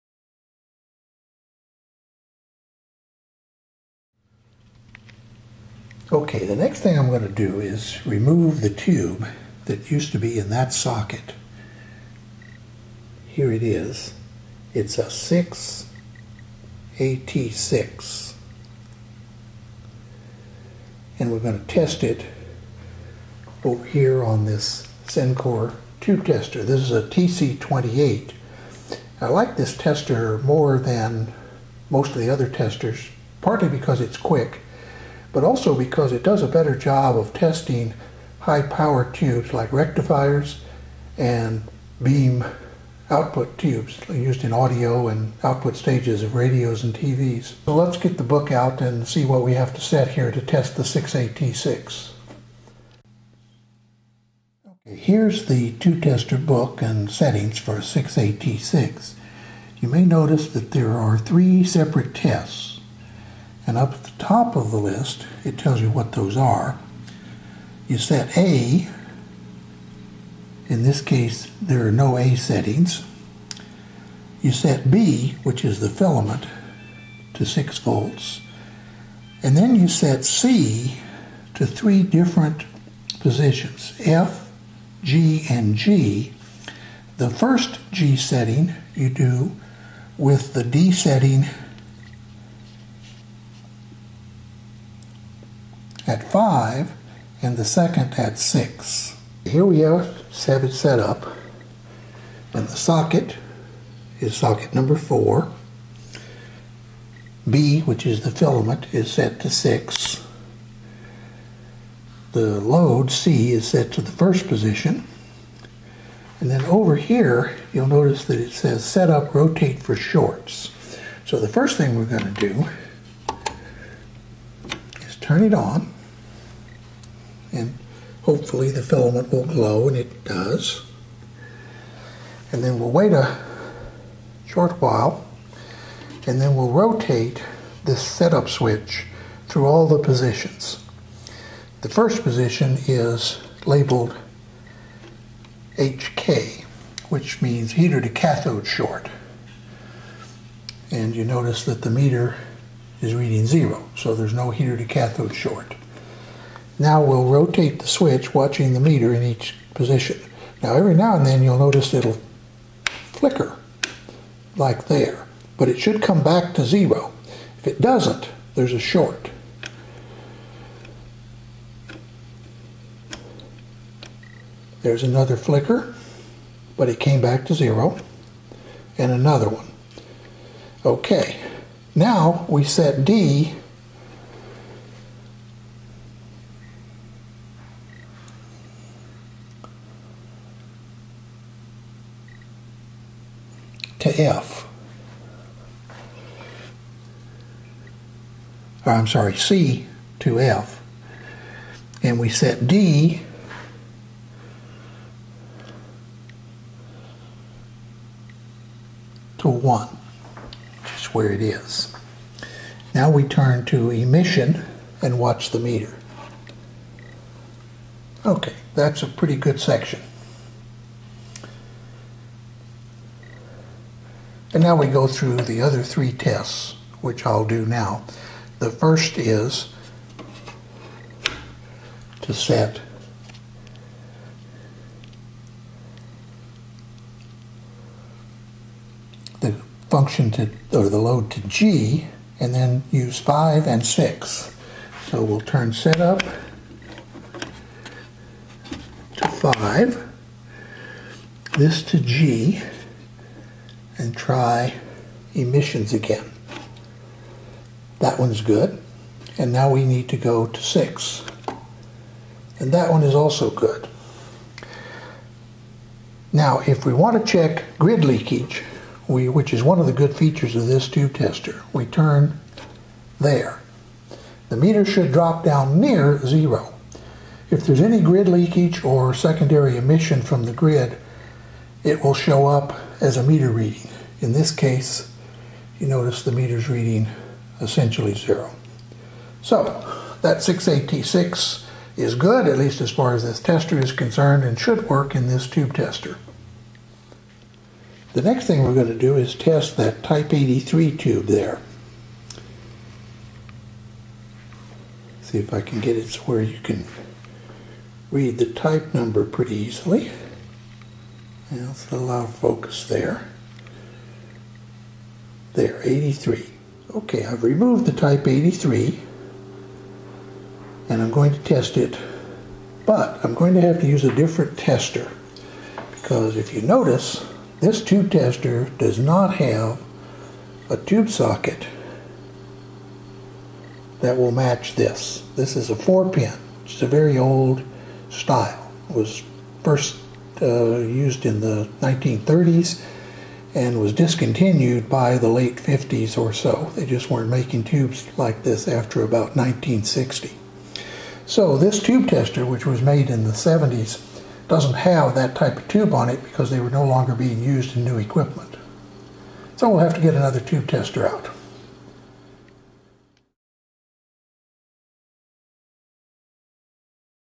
TTT026 BK 650 Pt2
Second video of the restoration of a B&K 650 Tube Tester. We begin to test the tubes used in the tester.